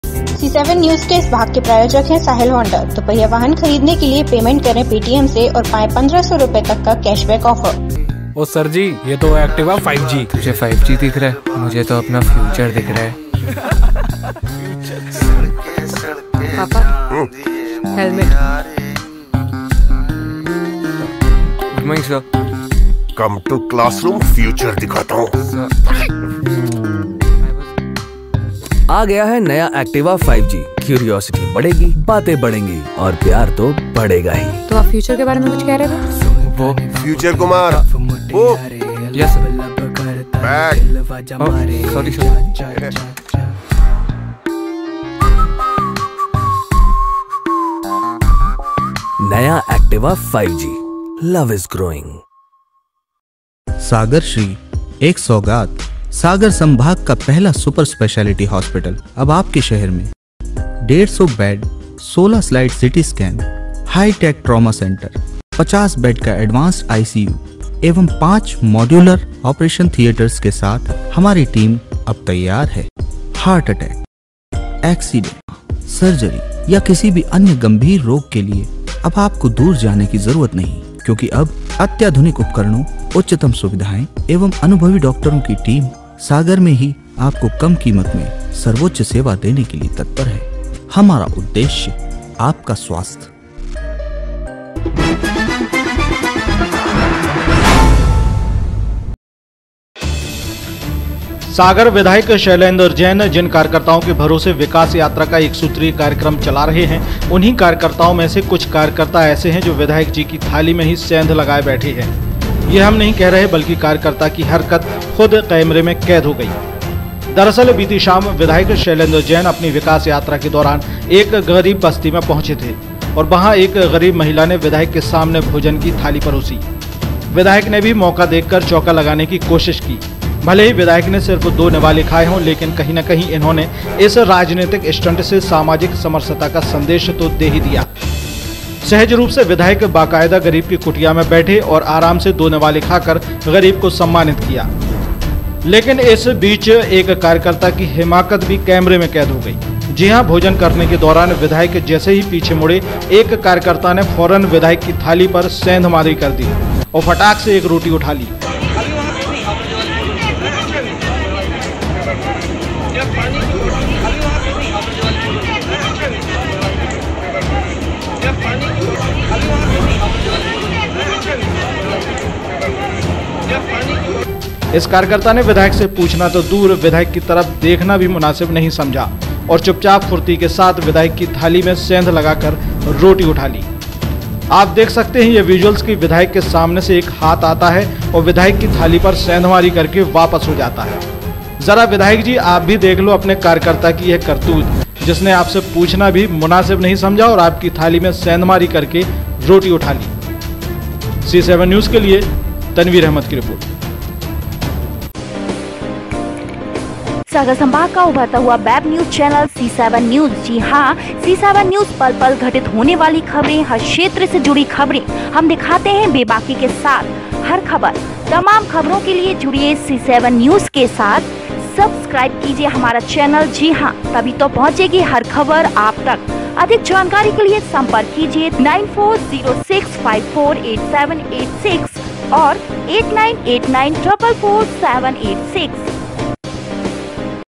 सागर : विधायक की थाली में सेंधमारी
सागर विधायक शैलेन्द्र जैन जिन कार्यकर्ताओं के भरोसे विकास यात्रा का एक सूत्रीय कार्यक्रम चला रहे हैं उन्हीं कार्यकर्ताओं में से कुछ कार्यकर्ता ऐसे हैं जो विधायक जी की थाली में ही सेंध लगाए बैठे हैं । ये हम नहीं कह रहे बल्कि कार्यकर्ता की हरकत खुद कैमरे में कैद हो गई ।  दरअसल बीती शाम विधायक शैलेन्द्र जैन अपनी विकास यात्रा के दौरान एक गरीब बस्ती में पहुॅंचे थे और वहां एक गरीब महिला ने विधायक के सामने भोजन की थाली परोसी, विधायक ने भी मौका देखकर चैका लगाने की कोशिश की । भले ही विधायक ने सिर्फ दो निवाले खाये हों लेकिन कहीं न कहीं इन्होंने इस राजनैतिक स्टंट से सामाजिक समरसता का संदेश तो दे ही दिया । सहज रूप से विधायक बाकायदा गरीब की कुटिया में बैठे और आराम से दो निवाले खाकर गरीब को सम्मानित किया । लेकिन इस बीच एक कार्यकर्ता की हिमाकत भी कैमरे में कैद हो गई । जी हां भोजन करने के दौरान विधायक जैसे ही पीछे मुड़े एक कार्यकर्ता ने फौरन विधायक की थाली पर सेंधमारी कर दी और फटाक से एक रोटी उठा ली । इस कार्यकर्ता ने विधायक से पूछना तो दूर विधायक की तरफ देखना भी मुनासिब नहीं समझा और चुपचाप फुर्ती के साथ विधायक की थाली में सेंध लगाकर रोटी उठा ली । आप देख सकते हैं यह वीजुअल की विधायक के सामने से एक हाथ आता है और विधायक की थाली पर सेंधमारी करने वापस हो जाता है । जरा विधायक जी आप भी देख लो अपने कार्यकर्ता की यह करतूत जिसने आपसे पूछना भी मुनासिब नहीं समझा और आपकी थाली में सेंधमारी करके रोटी उठा ली । सीसेवन न्यूज के लिए तनवीर अहमद की रिपोर्ट